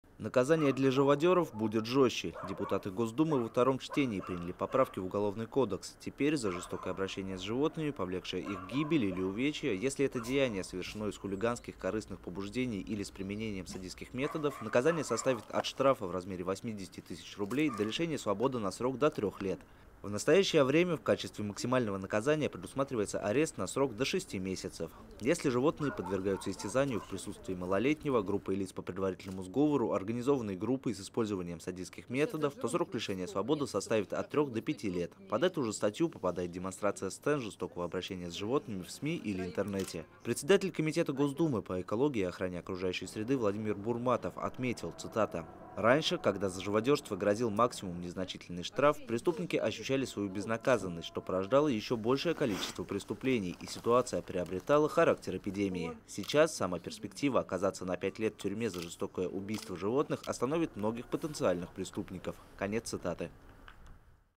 Наказание для живодёров будет жёстче
8 декабря 2017 | Депутаты Госдумы во втором чтении приняли поправки в Уголовный кодекс. Теперь «за жестокое обращение с животными, повлекшее их гибель или увечье, если это деяние совершено из хулиганских, корыстных побуждений или с применением садистских методов» наказание составит от штрафа в размере 80 тыс. рублей до лишения свободы на срок до трёх лет.